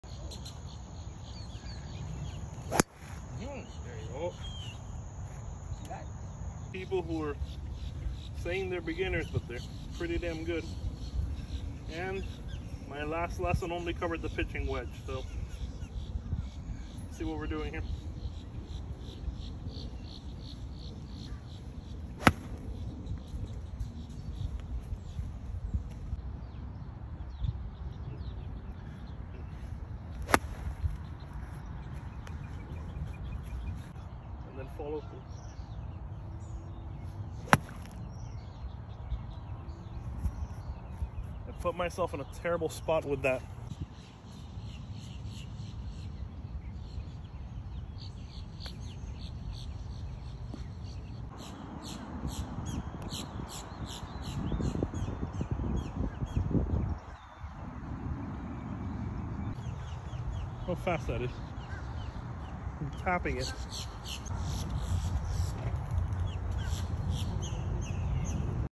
My First Golf Hole After My Lesson at Davie Golf Club This Weekend by TonyTriesGolf
I’m a big fan of Davie Golf Club. I played the silver tees and my first hole was one of two triple bogeys I had. Check out my first hole of golf after my first lesson.